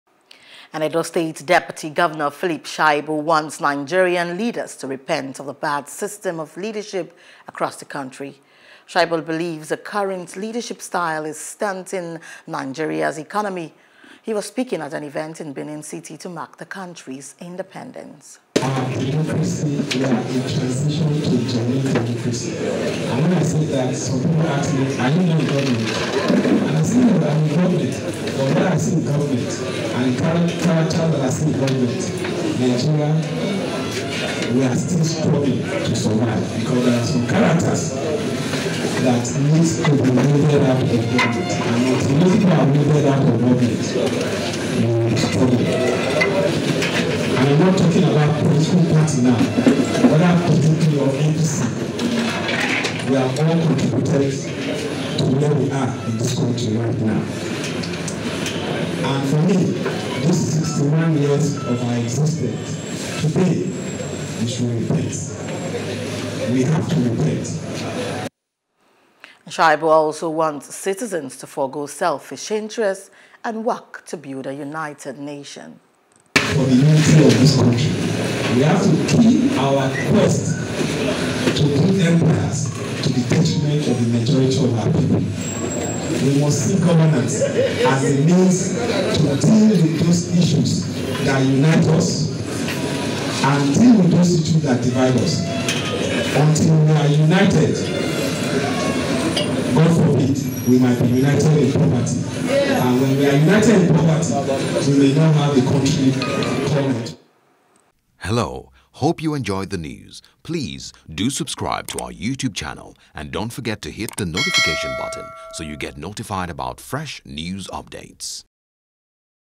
Edo State Deputy Gov. Preaches Selflessness | NEWS
Edo state deputy governor, Philip Shaibu wants Nigerian leaders to repent of the bad system of leadership across the country.